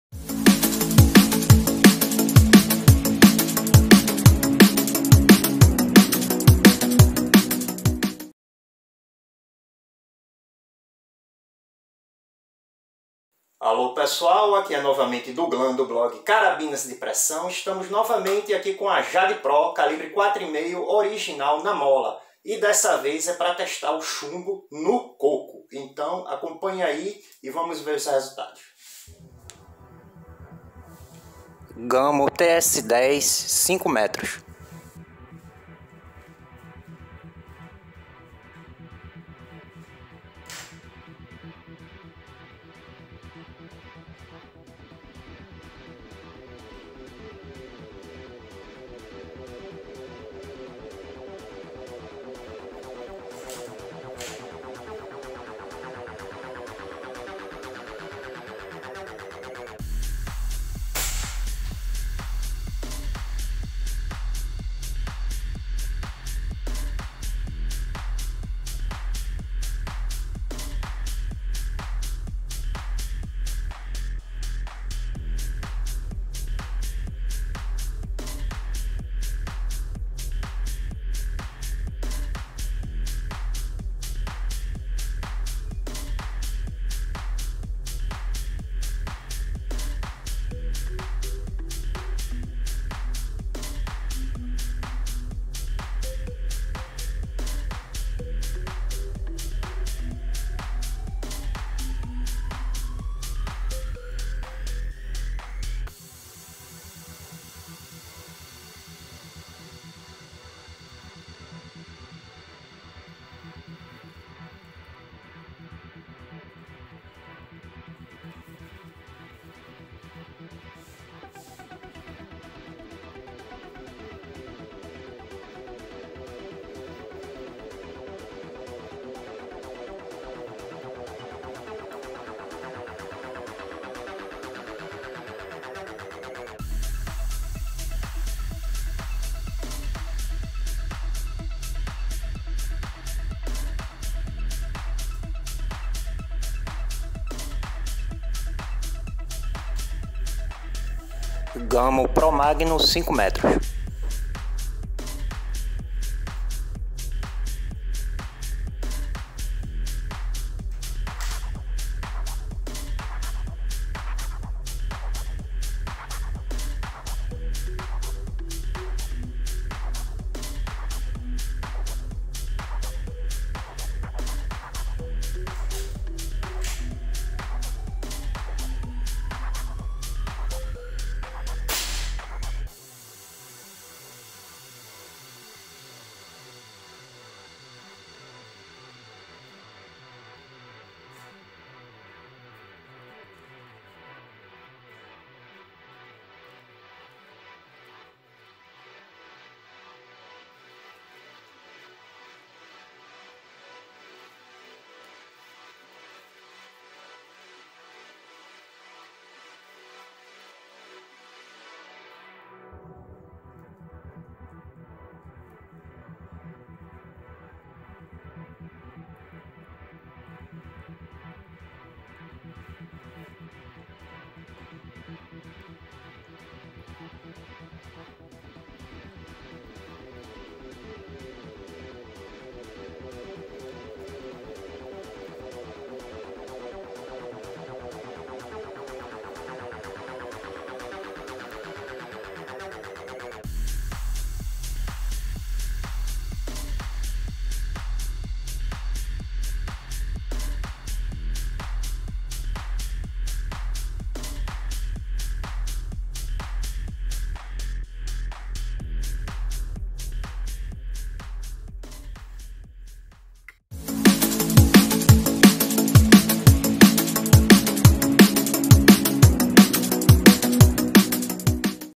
Jade Pro 4.5 - Transfixa Coco Seco
Carabina CBC Jade Pro 4.5 Transfixa coco seco com chumbo Gamo TS-10
Testando com Gamo Pro Magnum Causa grande fissura

Informações Técnicas:
Calibre: 4.5 mm (.177);
Coronha: Polímero de alta resistência;
Engatilhamento: Alavanca acionada pelo basculamento do cano;
Trava de segurança: Automática;
Velocidade Inicial*: 305m/s(1000 FPS);
Comprimento do cano: 40 cm;
Comprimento total: 112 cm;
Peso: 2,672 kg;

Technical information:
Caliber: 4.5 mm (.177);
Stock: High strength polymer;
Cocking: Lever activated by tilting the barrel;
Security lock: Automatic;
Initial Speed: 305m / s (1000 FPS);
Barrel length: 40 cm;
Total length: 112 cm;
Weight: 2,672 kg;